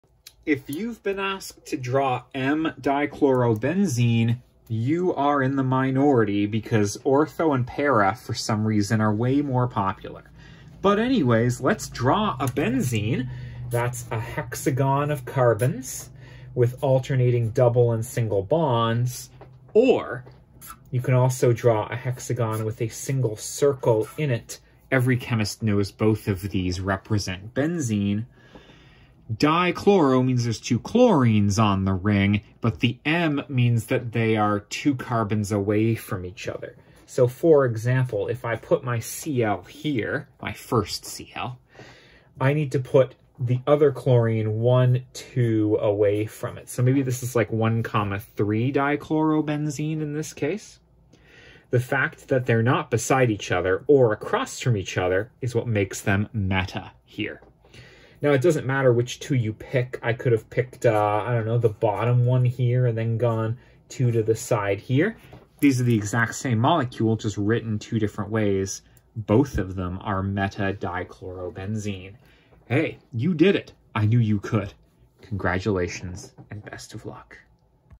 Draw m-dichlorobenzene (meta)
meta-dichlorobenzene has chlorines on carbons 1 and 3 of the hexagon.